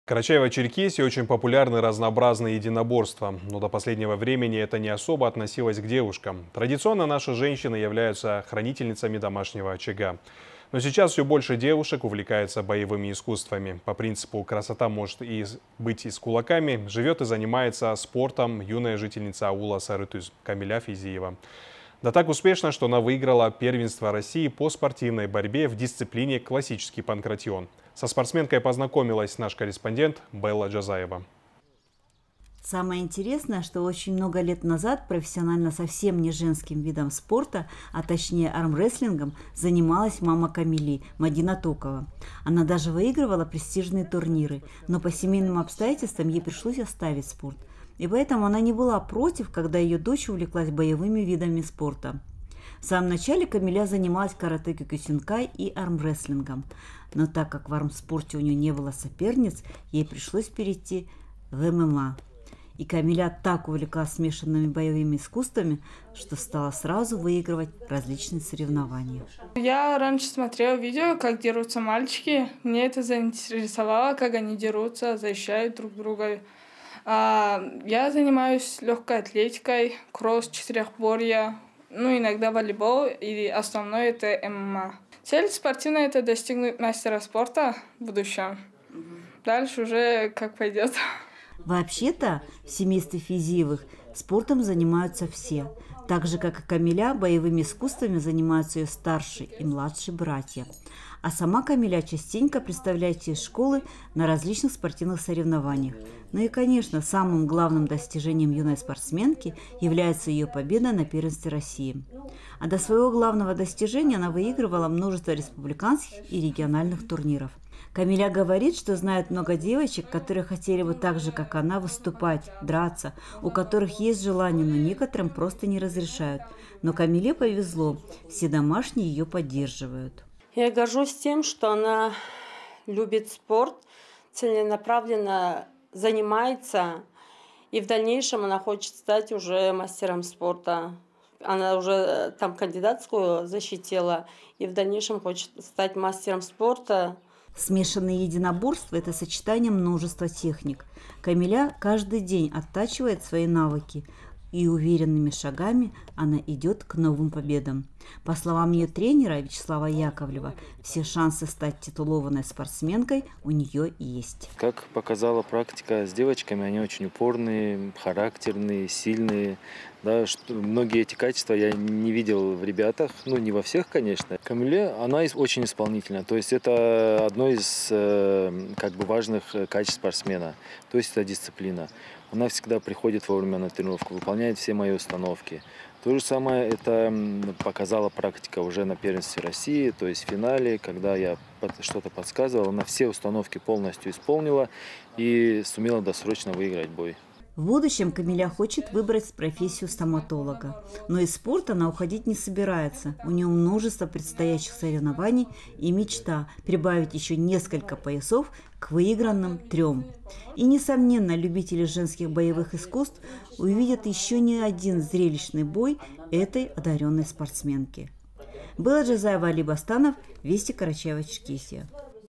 Красота может быть и с кулаками
В Карачаево-Черкесии очень популярны разнообразные единоборства, но до последнего времени это не особо относилось к девушкам. Традиционно наши женщины являются хранительницами домашнего очага. Но сейчас все больше девушек увлекаются боевыми искусствами. По принципу "красота может быть и с кулаками" живет и занимается спортом юная жительница аула Сары-Тюз Камиля Физиева, да так успешно, что она выиграла первенство России по спортивной борьбе в дисциплине классический панкратион.